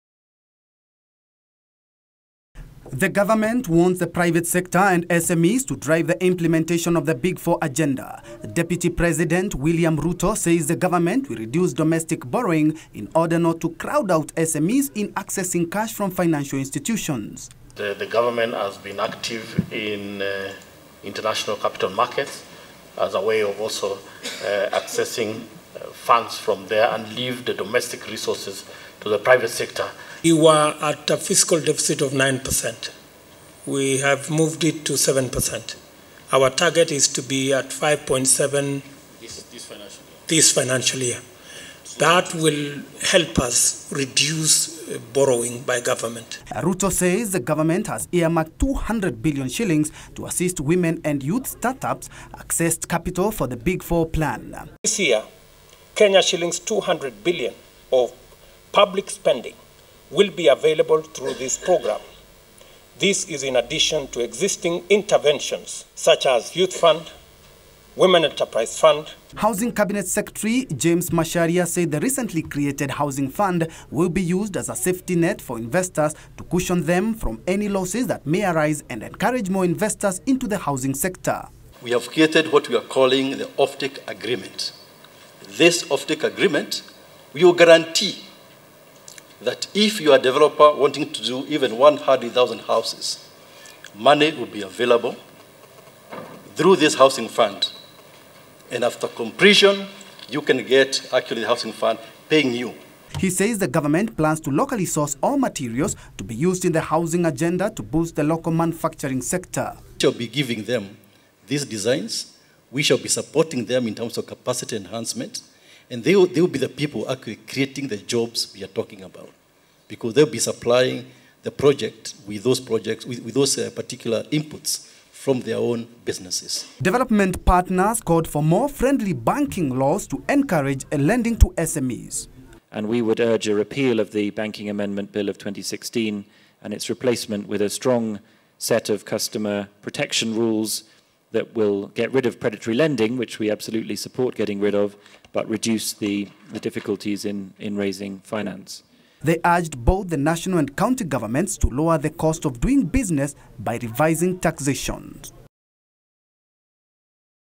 Gov't to reduce domestic borrowing
The government will reduce domestic borrowing to allow the private sector access capital  to invest in the big four agenda. Deputy President William Ruto says, the government has earmarked 200 billion shillings for financing women and youth startups as the government seeks to leverage on SMEs in implementation of the big four agenda.